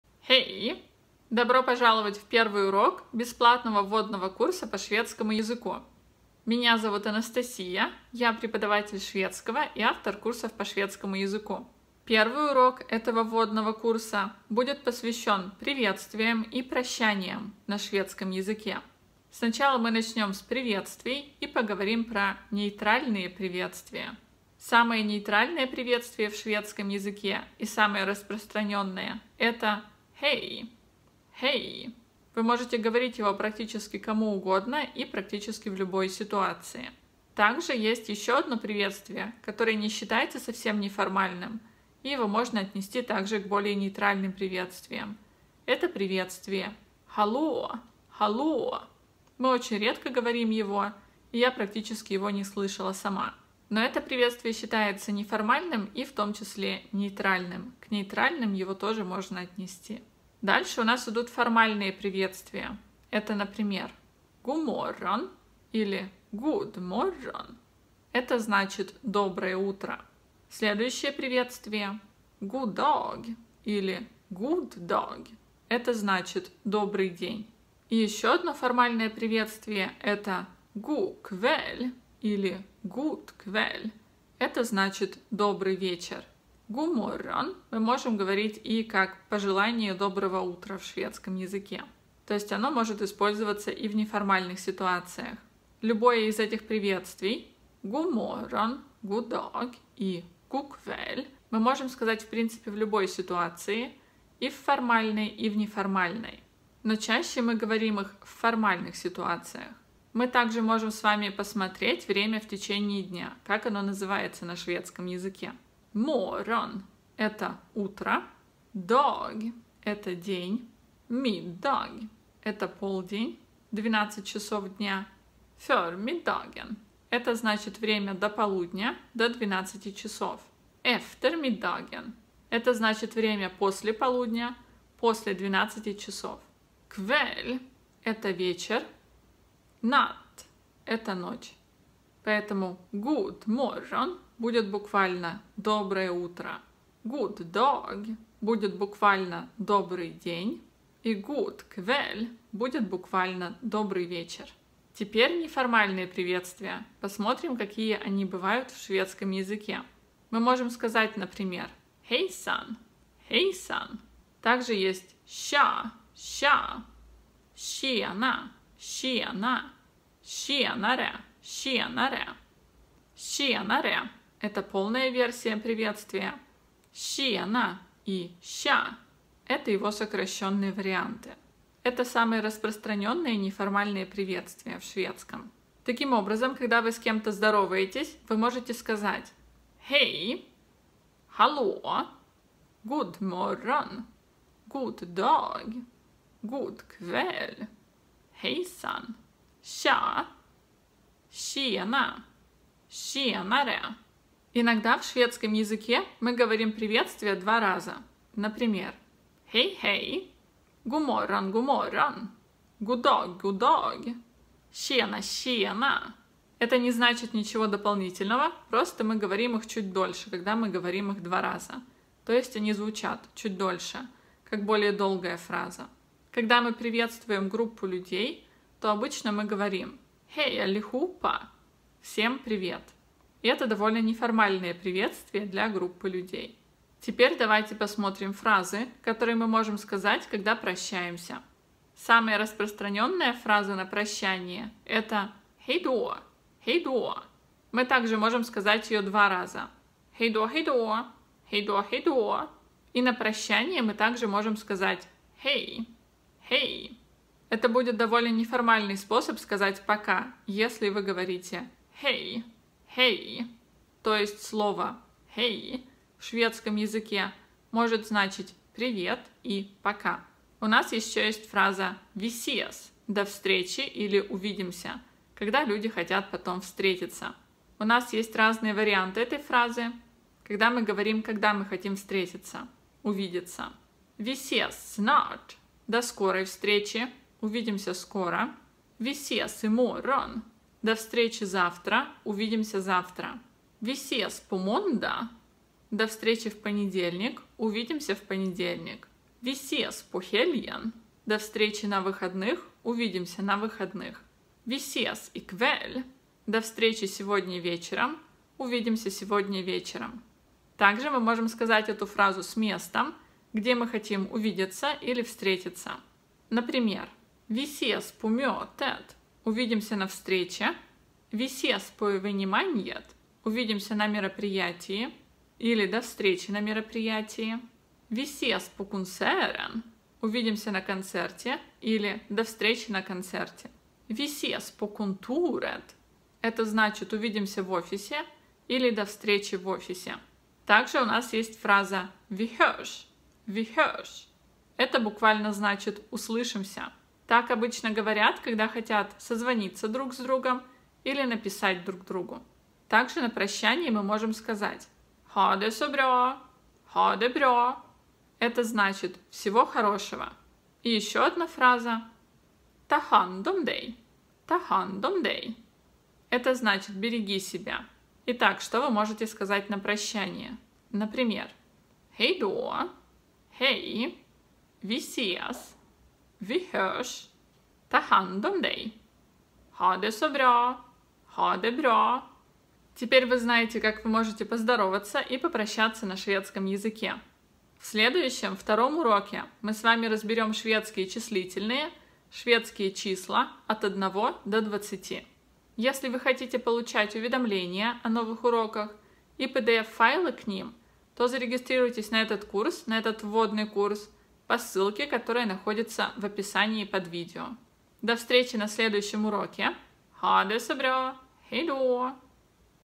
Урок 1. Приветствия и прощания: Вводный курс по шведскому языку | MinSwedish
В этом уроке Вы узнаете о том, как говорить приветствия и прощания на шведском языке.

Мы используем международную шкалу уровней CEFR.